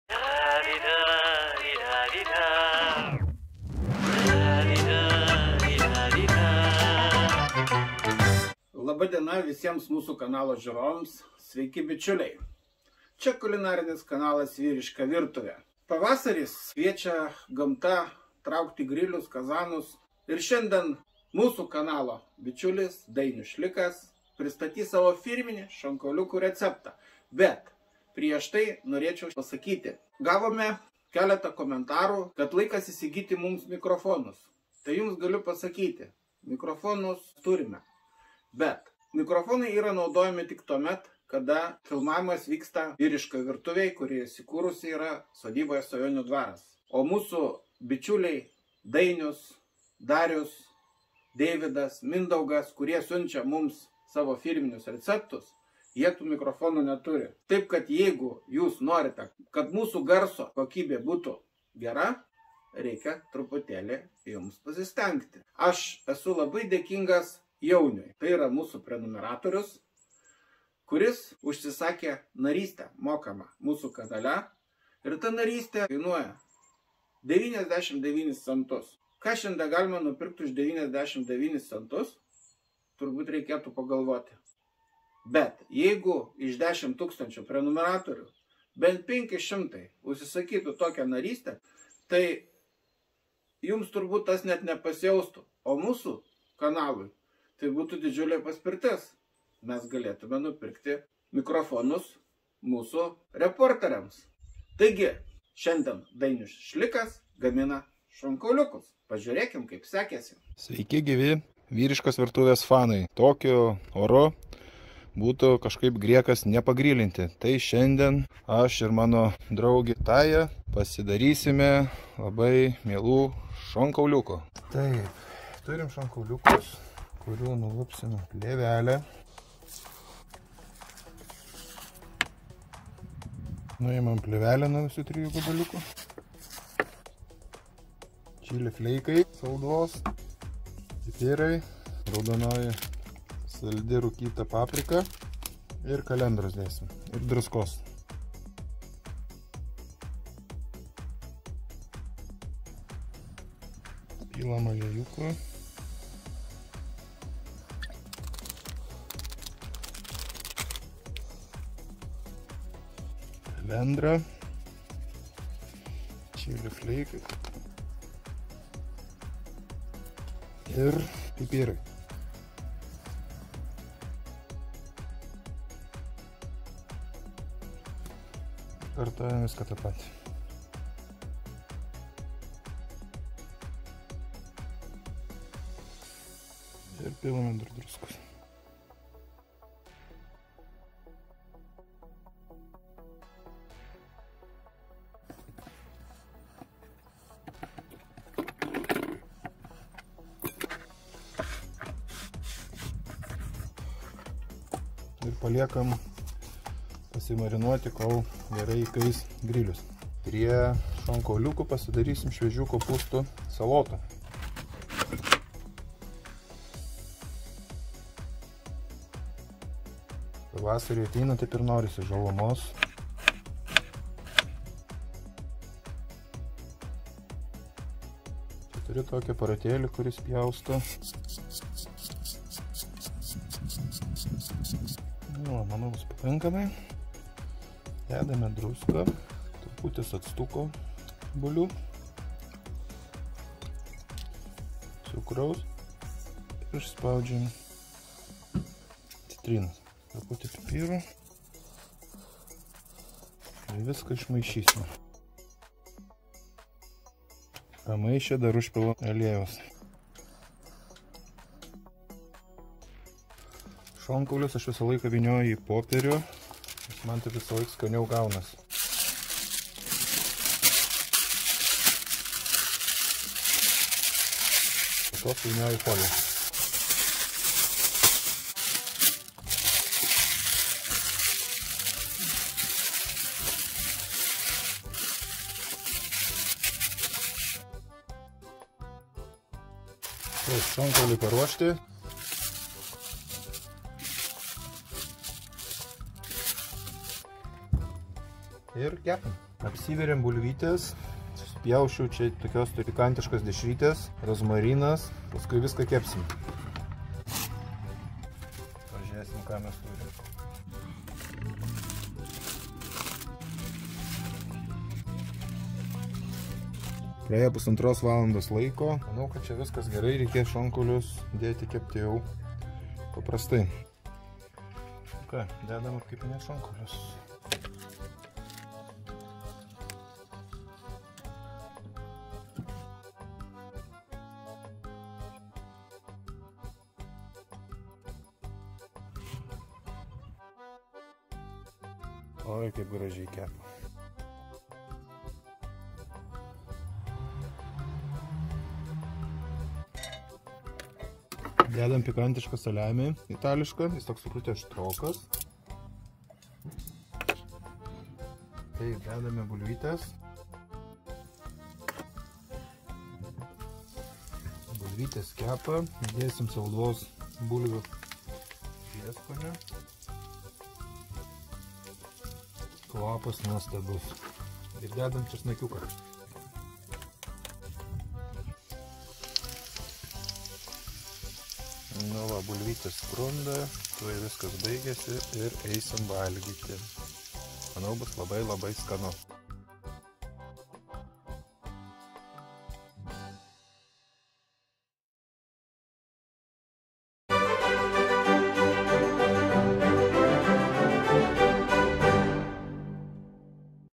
Šonkauliukai grilyje - firminis Dainiaus receptas
Dainiaus Šliko receptai! Šonkauliukai grilyje - vienas iš daugelio Dainiaus Šliko skanių receptų! Tobulai skanūs šonkauliukai! Vien į juos pažiūrėjus, seilė nutįsta iki žemės. Tokie šonkauliukai jūsų skonio receptorius veiks, kaip katinus veikia valerijono kvapas! Pradėję valgyti tokius šonkauliukus, negalėsite sustoti!
Jeigu neturite galimybių išsikepti tokius šonkauliukus grilyje, galite pamėginti orkaitėje, o dar įdomiau būtų kazane ant ugnies.
Vyriška virtuvė linki jums gero apetito!

Šonkauliukų grilyje receptas:
Kiaulienos šonkauliukai 2,5kg
Prieskoniai marinavimui: bbq prieskonių mišinys mėsai, čili dribsniai, juodi malti pipirai, malta saldi rūkyta paprika, malta kalendra, druska. Alyvuogių aliejus.
BBQ padažas glazūrai.
Garnyras:
1. Šviežių kopūstų salotos:
Šviežias kopūstas 1vnt
Morkos 2vnt
Druska
Obuolių actas
Cukrus
Citrinos sultys
Malti pipirai
Alyvuogių aliejus.
2. Grilio bulvytės:
Nepilnai išvirtos nedidelės bulvės 1,5kg
Itališka saliamio dešra
Rozmarino šakelė
Prieskonių mišinys bulvėms
Kelios česnako skiltelės.

Kviečiame į mūsų feisbuko grupę "Pasaulio patiekalai - gaminame kazane, grilyje"

Aliuminio konstrukcijos Alutech balkonams, langams, durims, pavėsinėms ir t.t.